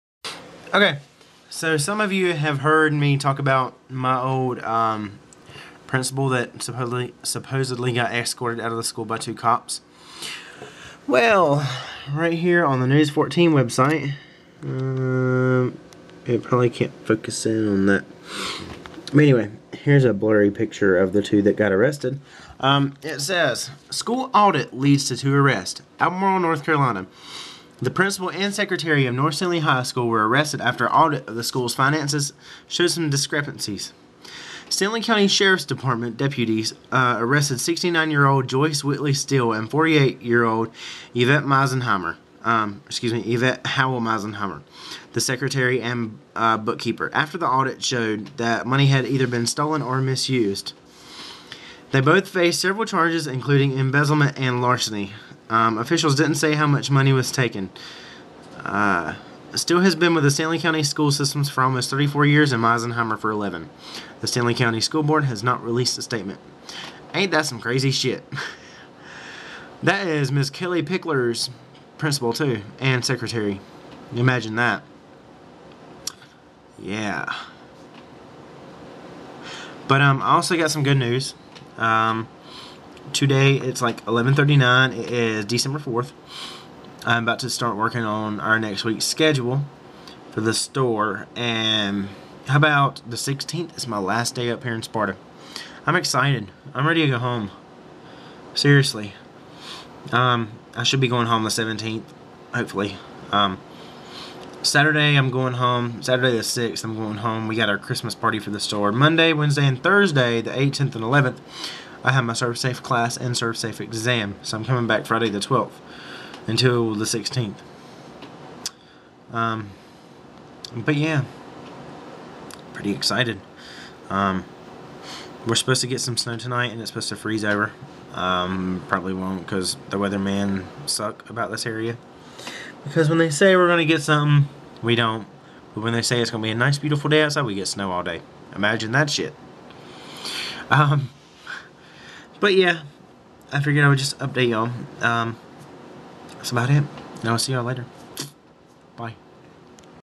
My Principal & Secretary Arrested
North Stanly High School's principal and secretary were arrested after an audit of the school funds showed money misused or stolen, unsure of the amount. Joyce Steele has been with the school system for 34 years, so you wouldn't have any clue how much total she's gotten away with. Yeah, same school as Kellie Pickler